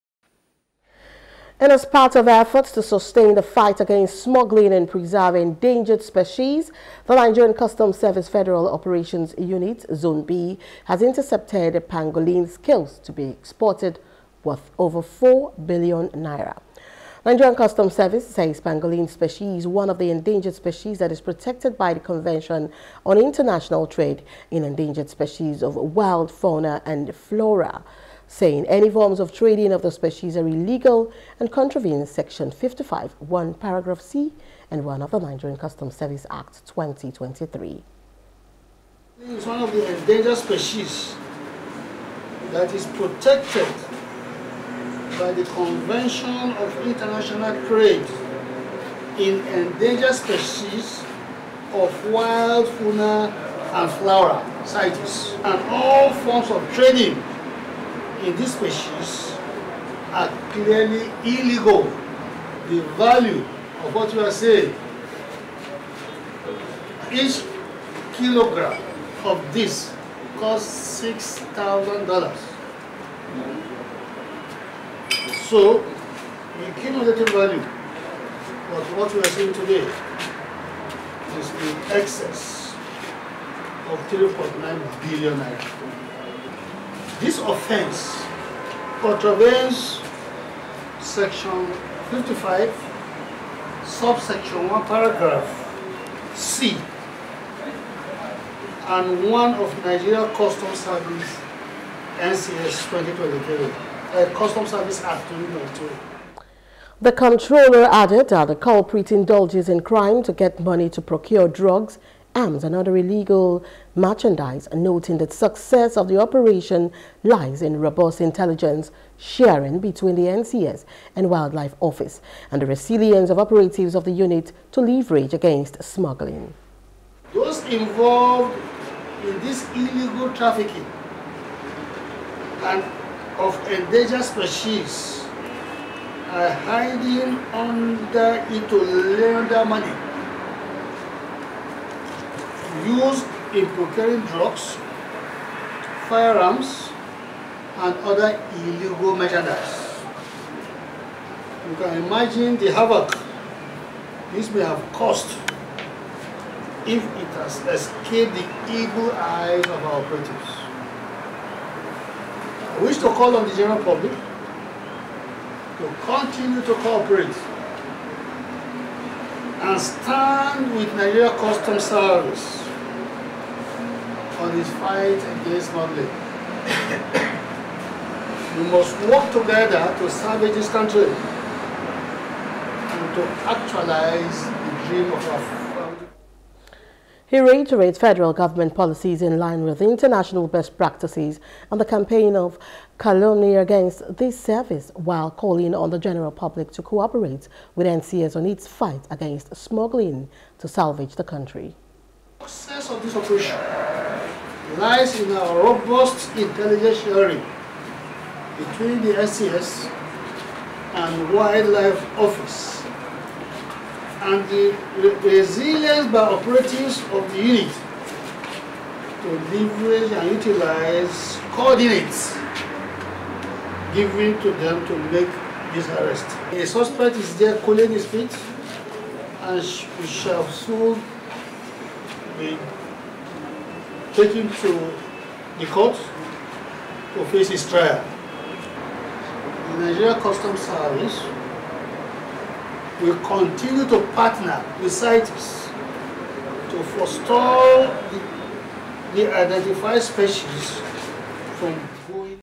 Custom: F.O.U. Zone Intercept Pangolin Scales For Export Worth 4 Billions
As part of effort to sustain the fight against smuggling and preserve endangered species, the Nigerian Customs Service Federal Operations Unit (FOU) Zone B, has intercepted Pangolin scales to be exported worth over N4 billion Naira.